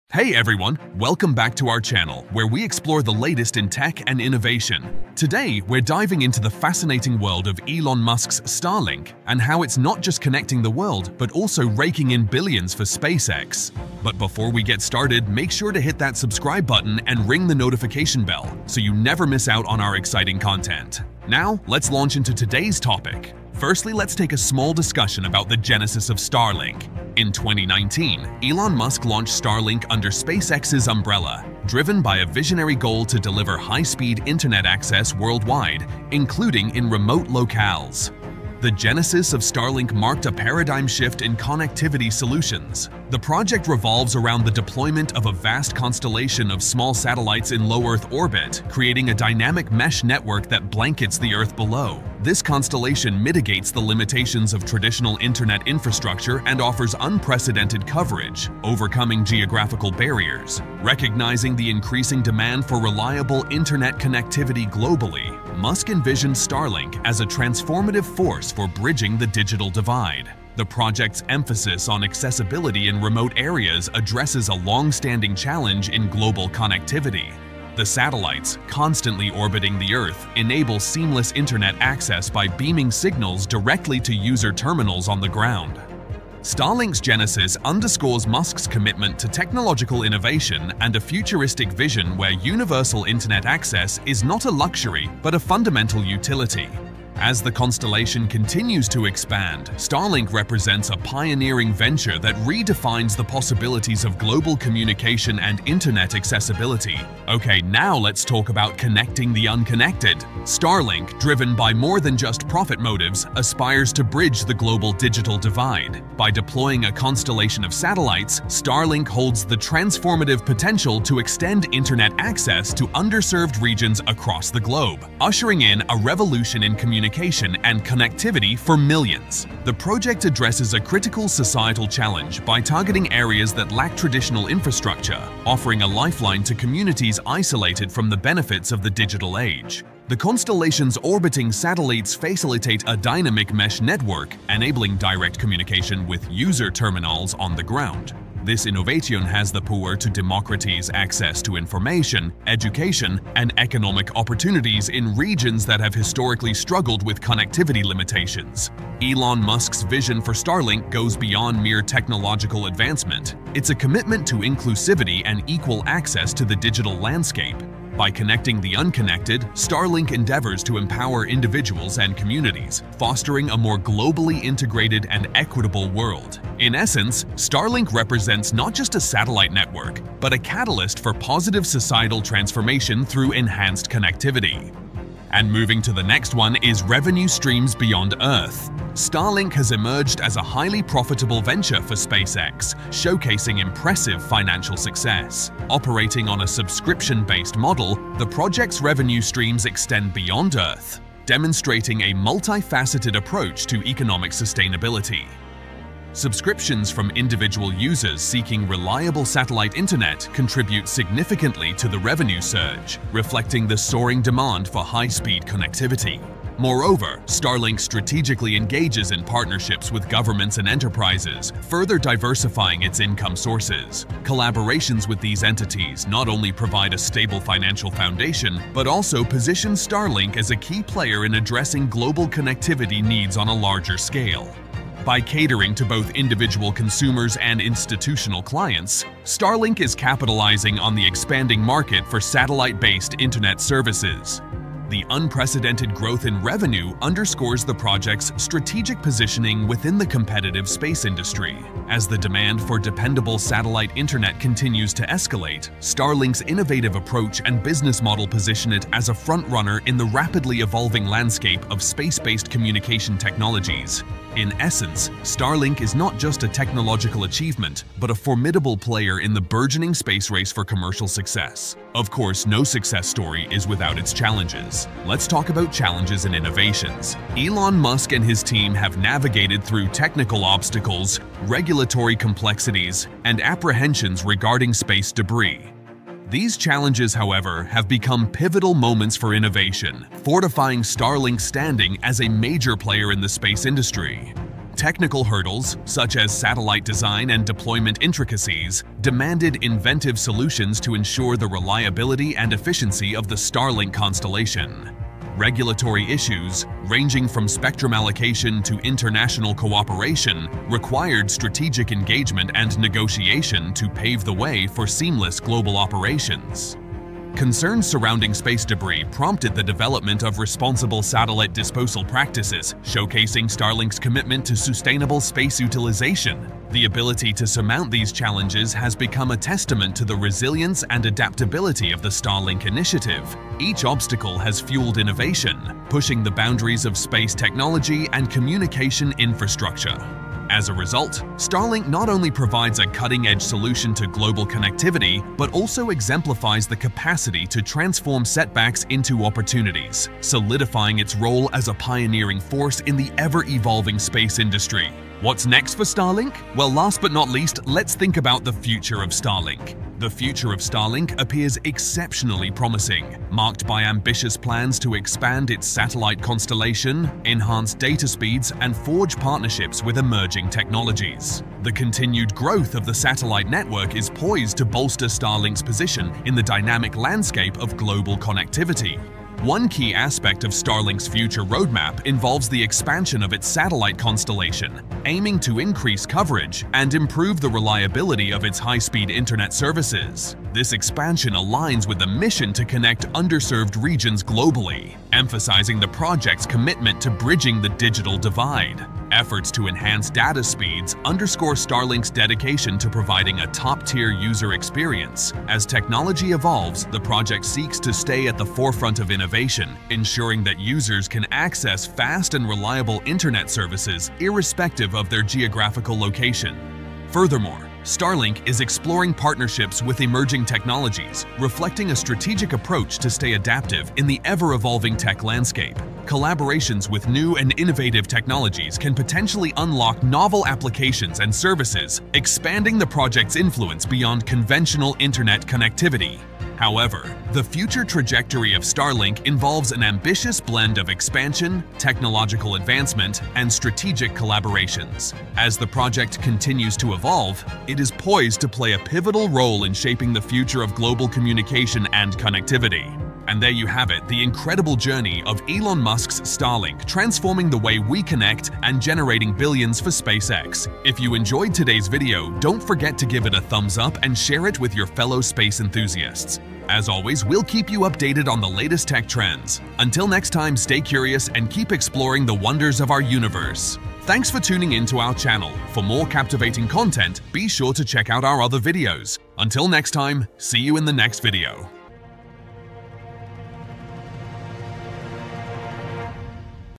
How Elon Musk’s Starlink Is Bringing In Billions For SpaceX
Elon Musk's new SpaceX project, Starlink, is taking off! Starlink is a network of satellites that will allow for high-speed internet access across the planet.

So far, Starlink has been successful, and SpaceX has already seen benefits from the project. Starlink has brought in billions of dollars for SpaceX, and that's only the beginning! In this video, we'll explore what Starlink is, how it works, and how it's benefiting SpaceX and the world.

📈 Why Starlink is a Game-Changer:
Explore the lucrative aspects that have contributed to the astronomical success of Starlink and understand why it's become a key player in the tech and space industries.

🌌 Join the Conversation:
Share your thoughts and predictions in the comments section! How do you think Starlink will continue to shape the world of technology and connectivity?